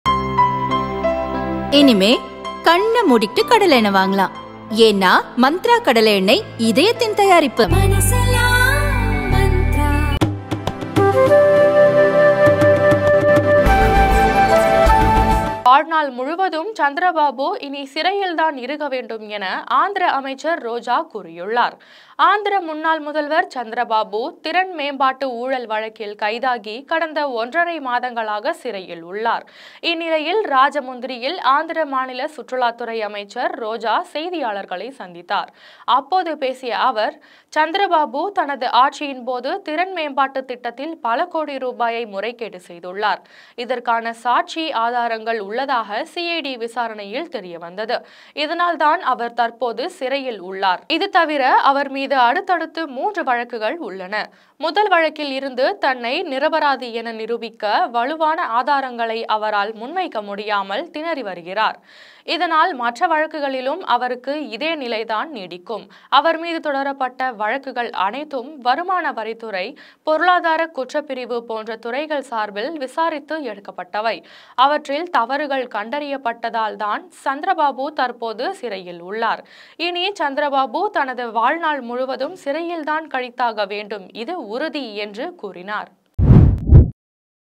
சந்திரபாபு வாழ்க்கையே சிறையில் தான் ! அமைச்சர் ரோஜா பரபர.| Chandrababu Naidu | Roja | Andhra Pradesh
சாணக்யா!